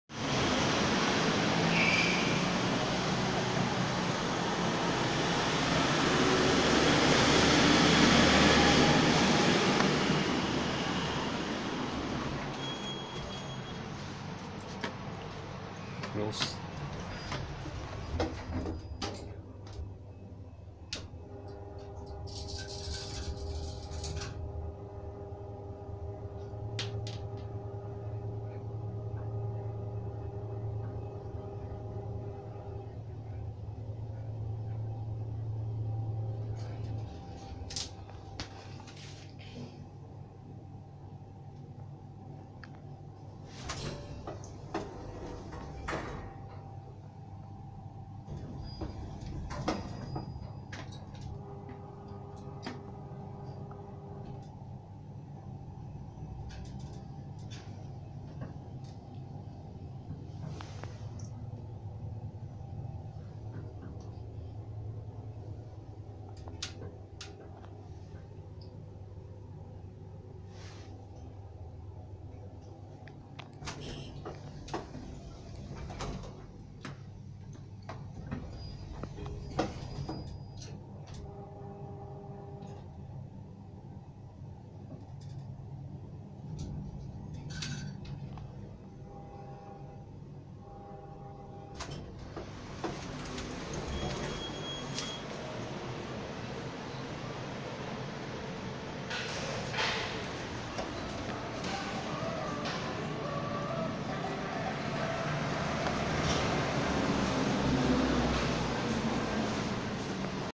TPI Building - Old Otis Spec 50 Lexan Lift
Located at 62 Cecil Street.

Taken on 29 July 2018. Pardon for poorer looking quality video as this is taken on my older phone at that time.